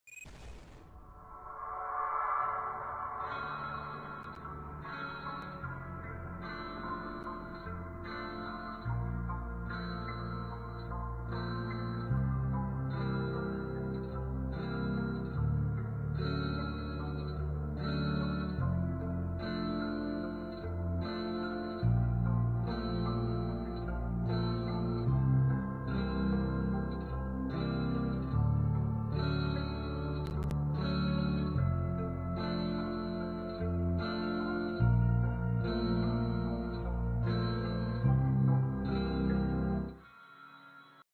Custom Timothy guiding light message!
So I made timothy a guiding light message because the little spider doesn't have one
(NOTE) Game might get taken down by LSplash